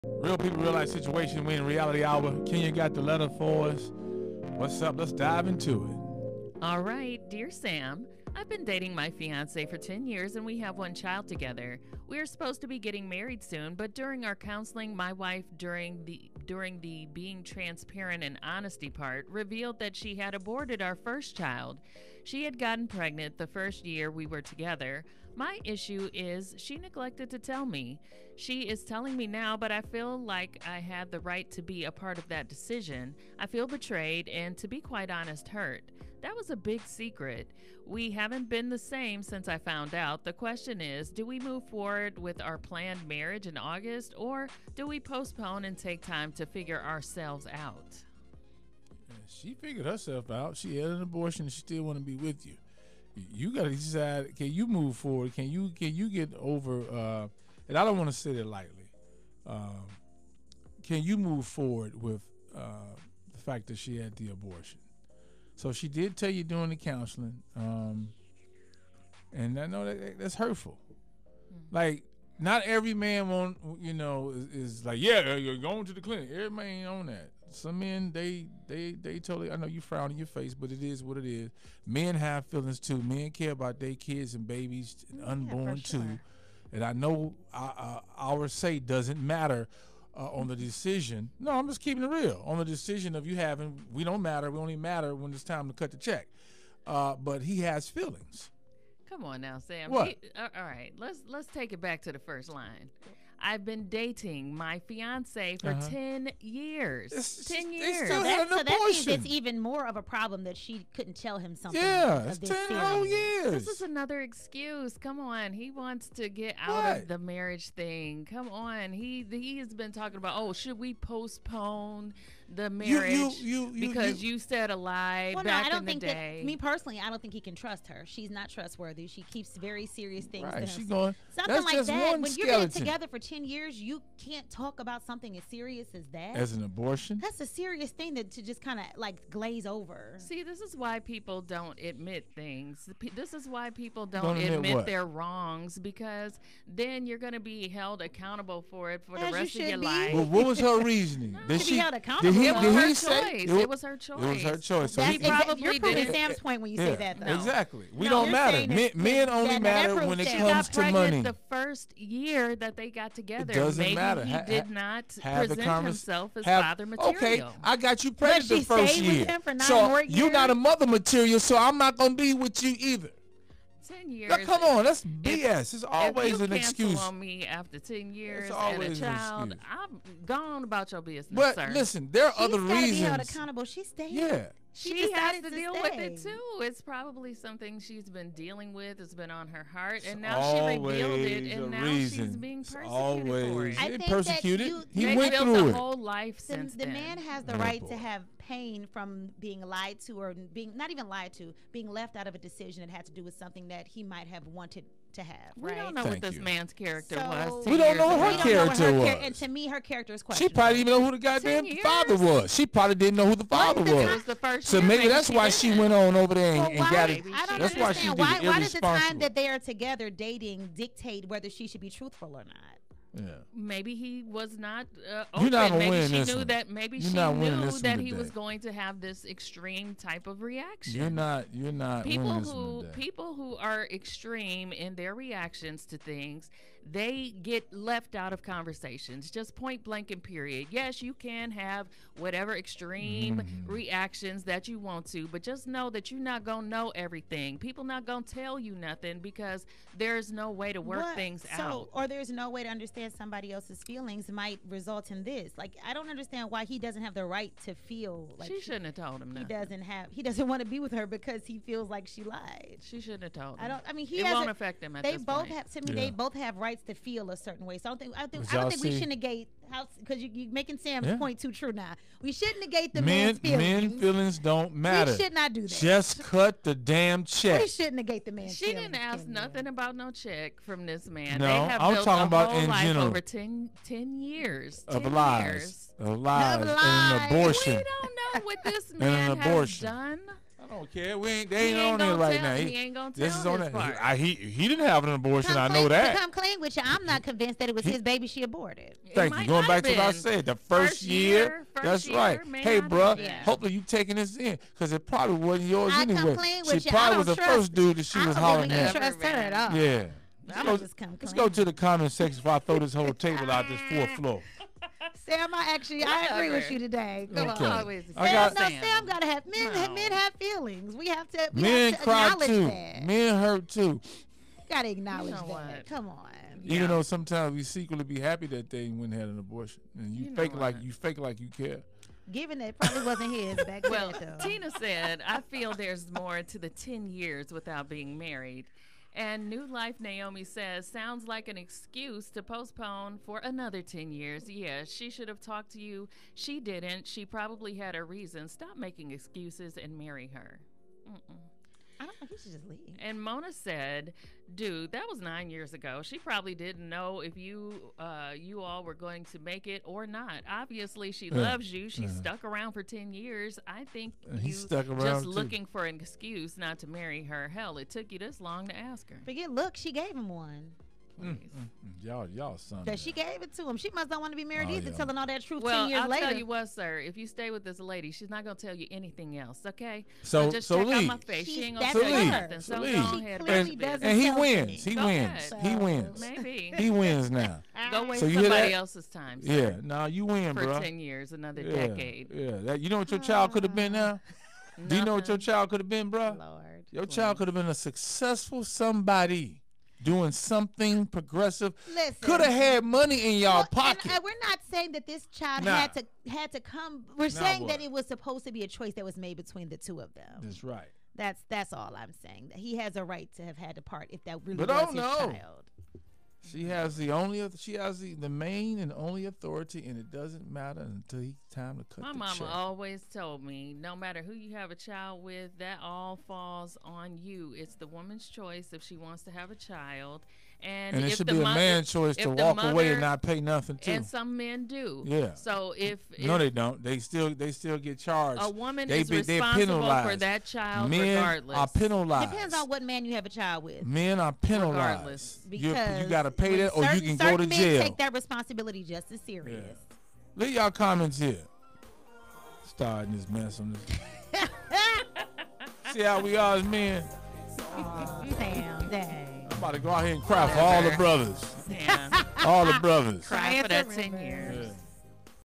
My fiancé had a secret abortion. Should I stay?
Dear Sam,
I have been dating my fiancé for 10 years and we have one child together. We are supposed to be getting married soon, but during our counseling my wife during the being transparent and honesty part revealed that she had aborted our first child. She had gotten pregnant the first year we were together. My issue is she neglected to tell me. She is telling me now but I feel I had a right to be apart of that decision. I feel betrayed and to be quite honest hurt. That was a big secret!! We haven’t been the same since I found out. The question is do we move forward with our planned marriage in August or do we postpone and take time to figure ourselves out?